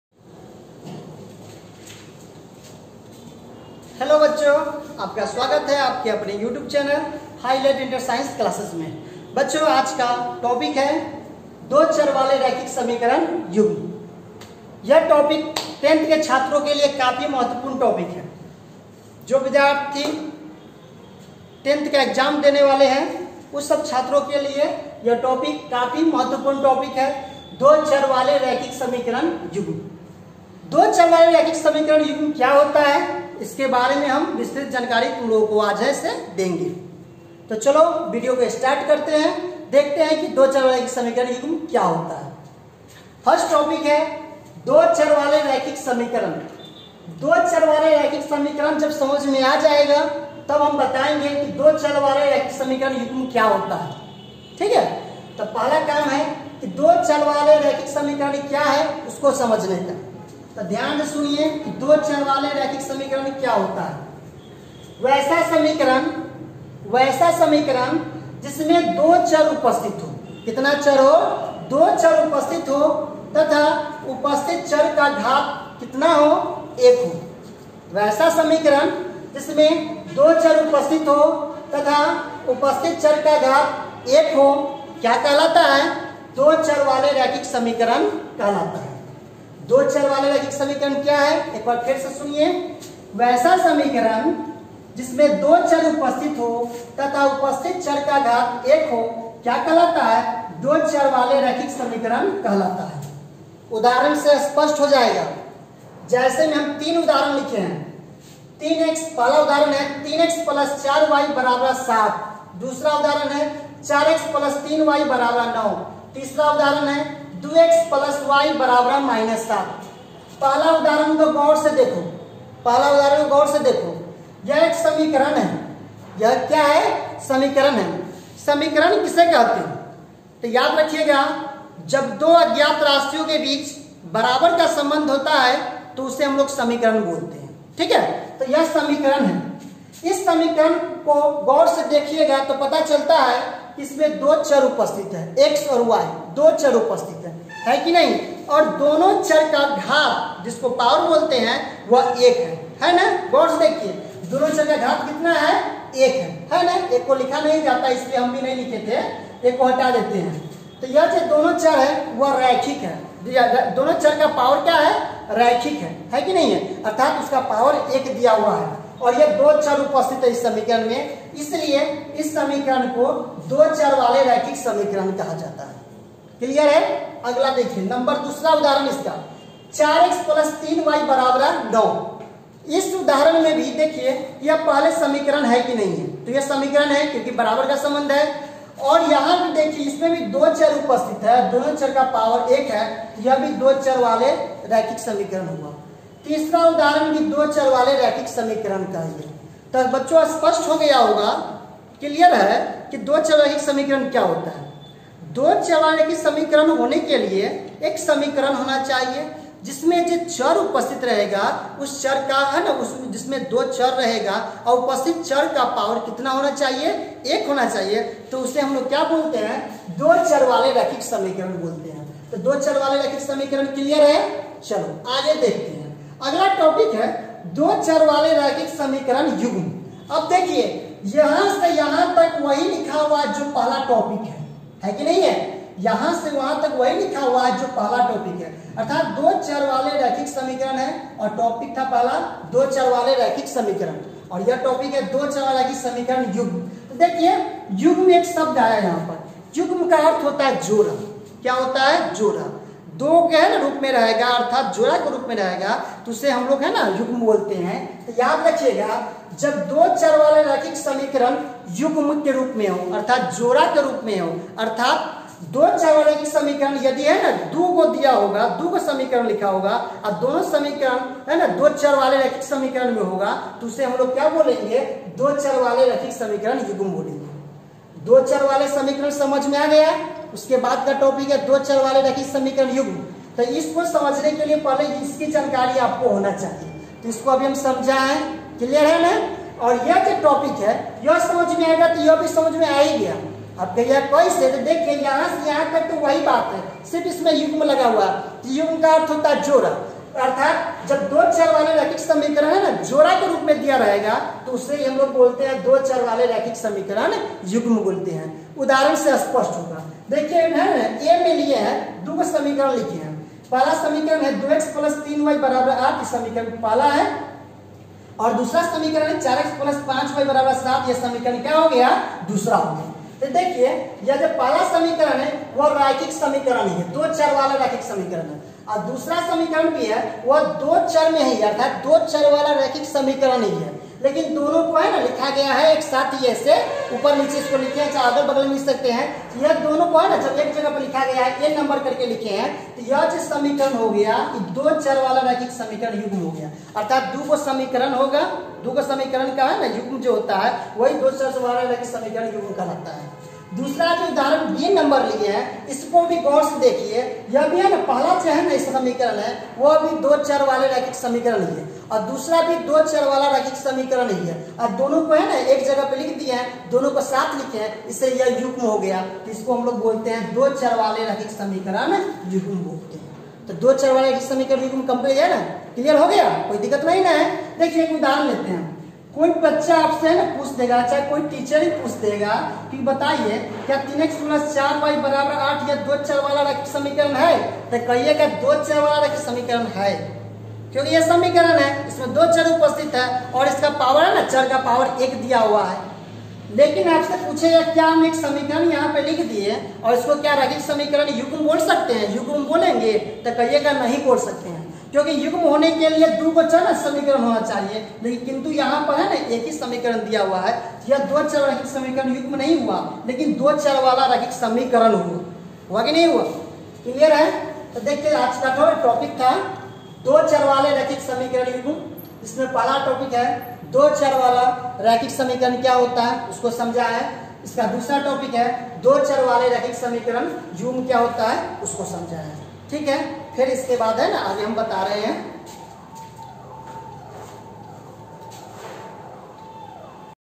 Linear equation pair having two variable (दो चर वाले रैखिक समीकरण युग्म) lecture 01 By -Dilip sir
HIGH LIGHT INTER SCIENCE CLASSES By-Dilip sir